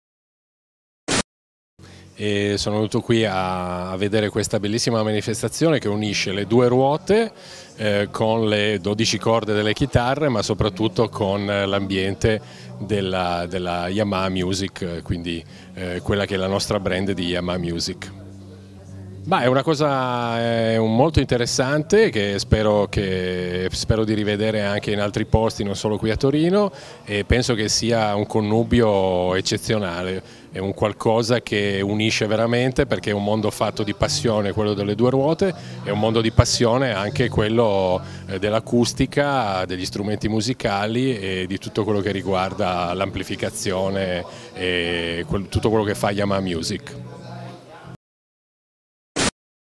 Fulvio Tucci, Yamaha Motor Europe
Anche il responsabile vendite Italia del canale off-road ha preso parte all'evento organizzato da Connessioni per unire la passione per la musica e quella per i motori